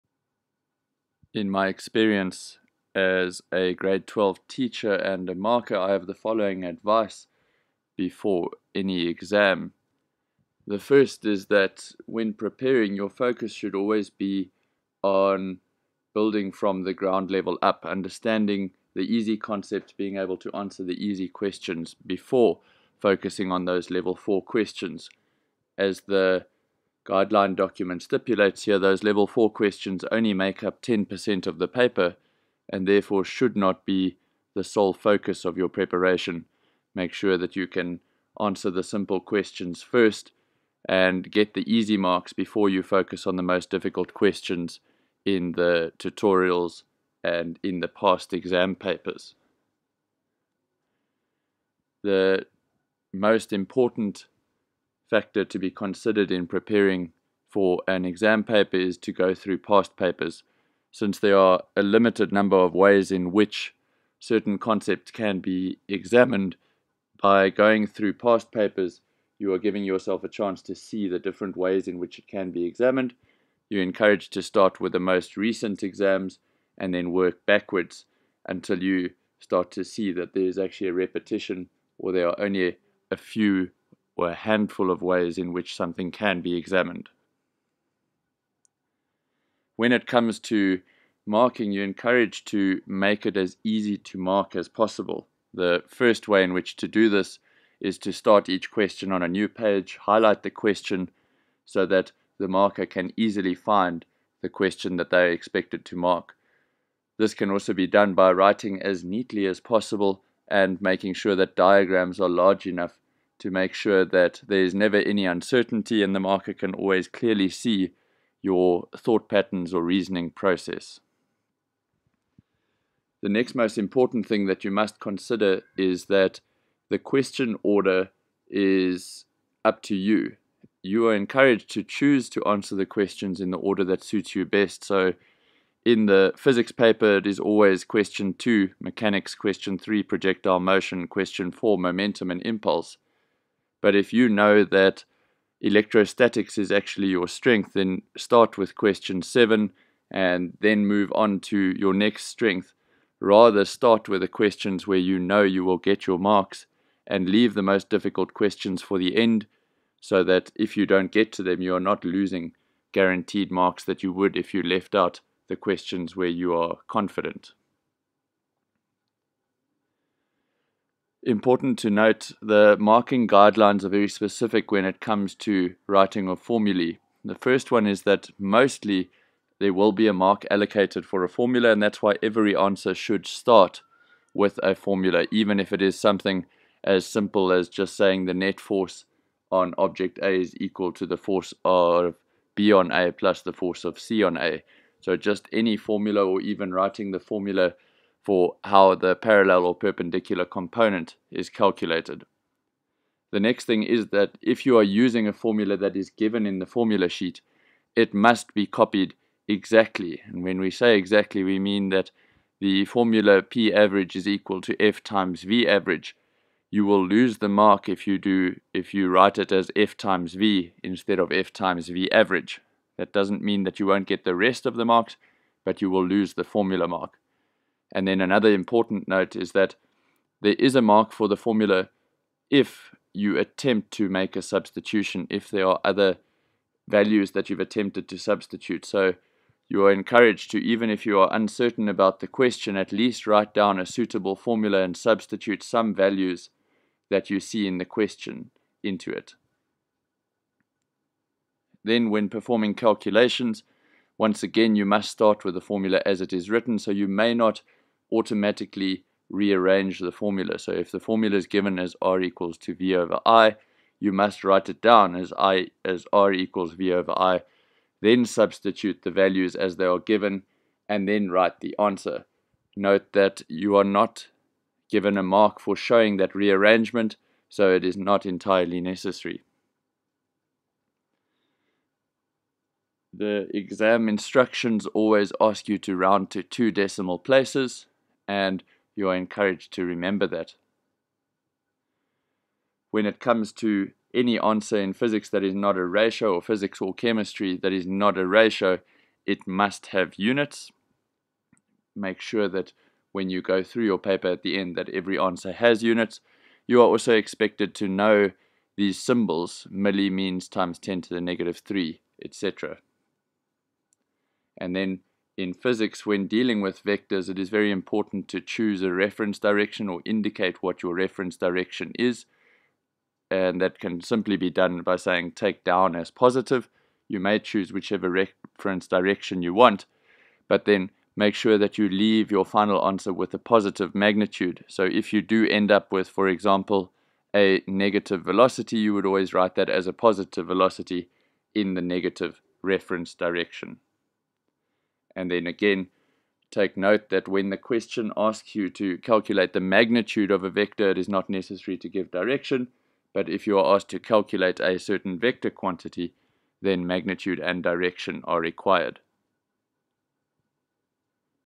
NSC Physical Sciences Exam Advice
This video outlines important factors to remember in preparing for and writing a NSC Physical Sciences examination.